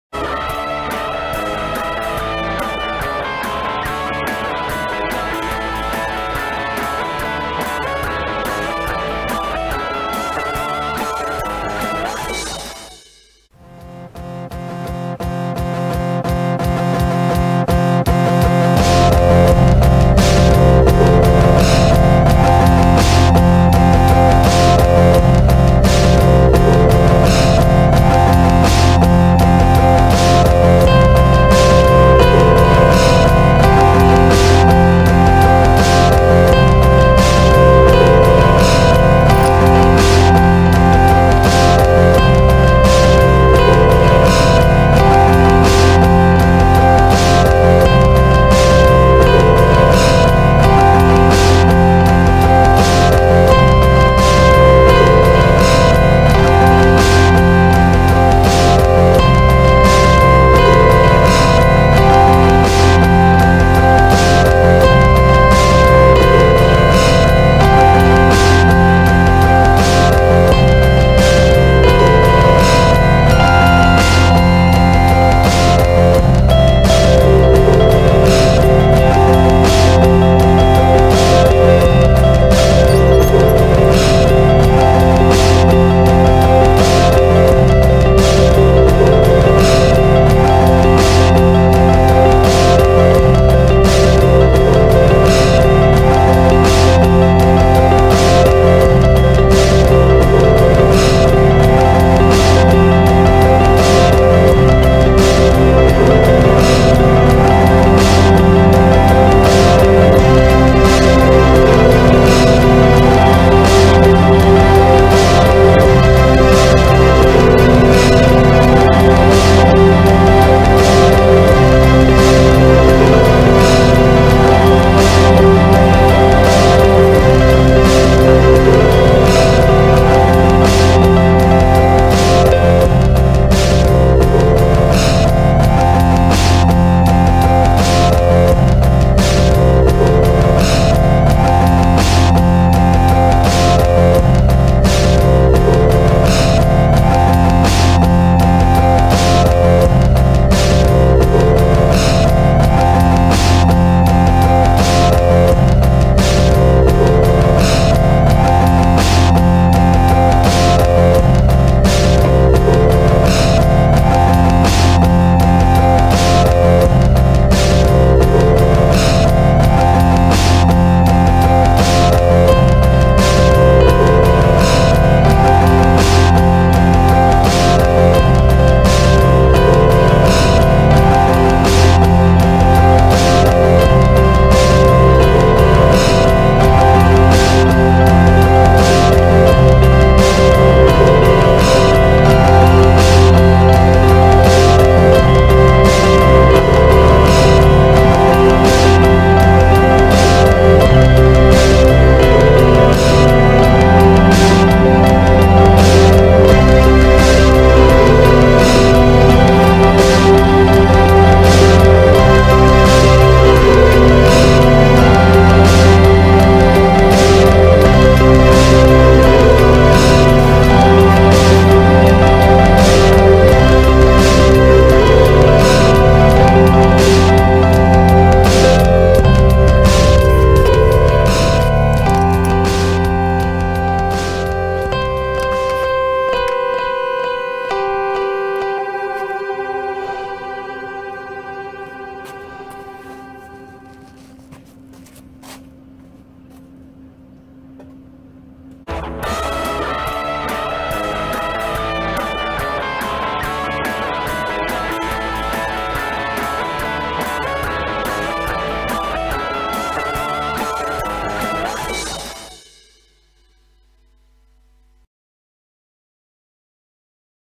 Celica Foglight Fix St204 6thGen
Easy to follow foglight bulb replacement on Toyota Celica St204 (GT,SX,SXR)